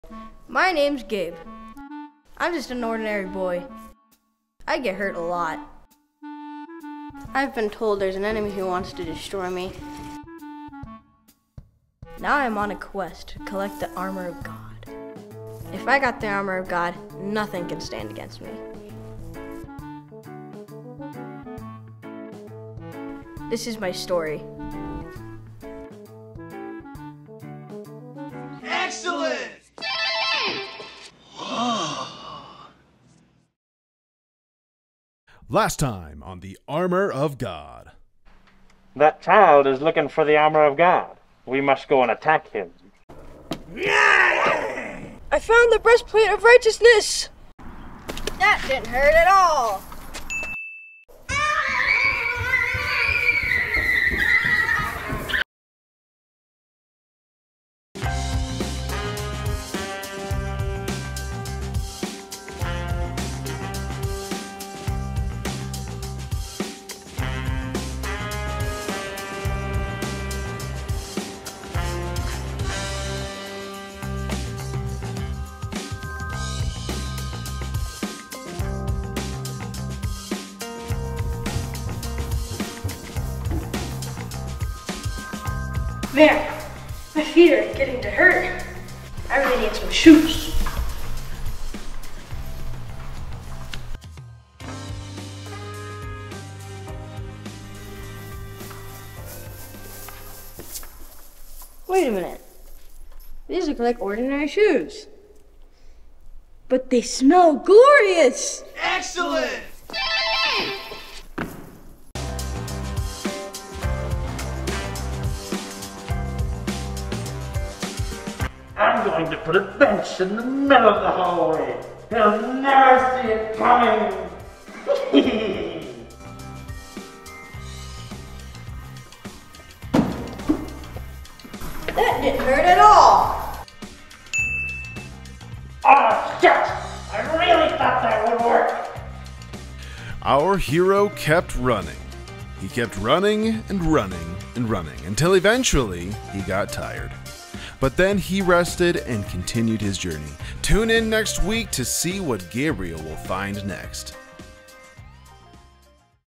Armor of God Skit Series - Chapter 3
Embark on an adventure with our hero, Gabriel, as he searches for the Armor of God! (This is a skit series we filmed for our Family Friendly series, "Armor of God" in July 2023.